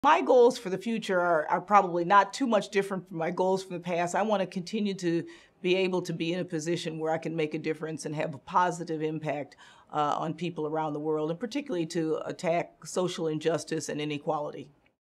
Nonprofit CEO: Goals - Dr Helene Gayle Career Girls Role Model
Watch Dr. Helene Gayle, CEO of CARE, share valuable career guidance and life advice with girls. Learn how to become an nonprofit CEO and watch her full interview